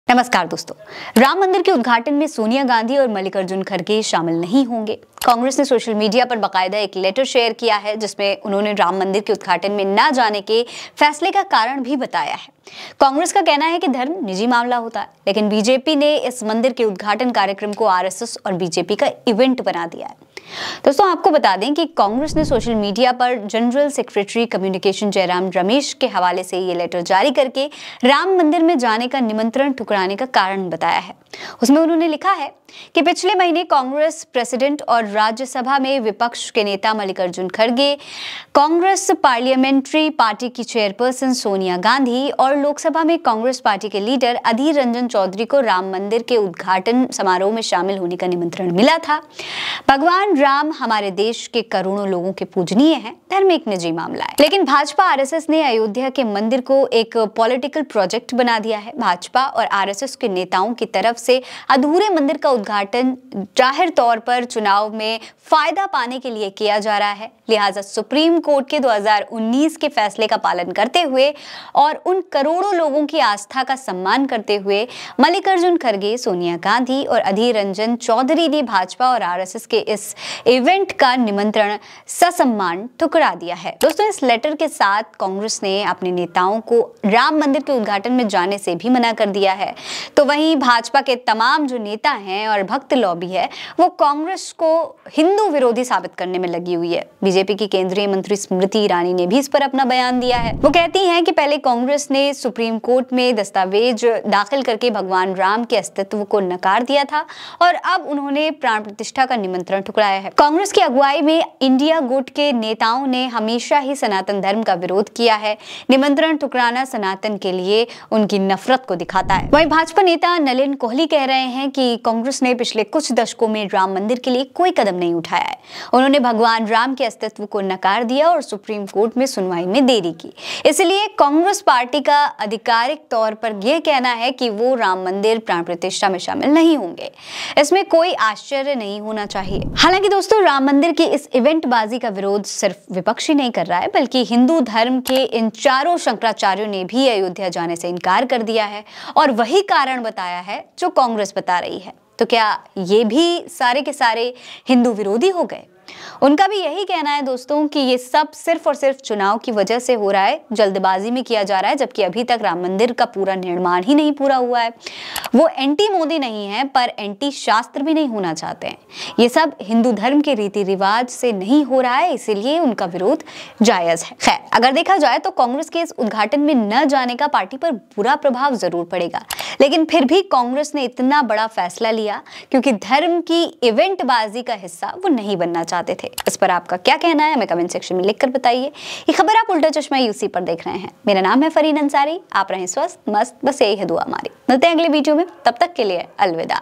BJP का अयोध्या वाला खेल बिगड़ गया | शंकराचार्यों के साथ खड़ी हो गईं Soniya Gandhi
Sonia Gandhi and Mallika Arjun Kharge will not attend the inauguration of Ram Temple. Congress has shared a letter on social media, in which it has given the reason for the decision of not attending the inauguration of Ram Temple. In this, Congress has written that Religion is a personal matter, but BJP has made the inauguration program of this temple its own RSS event.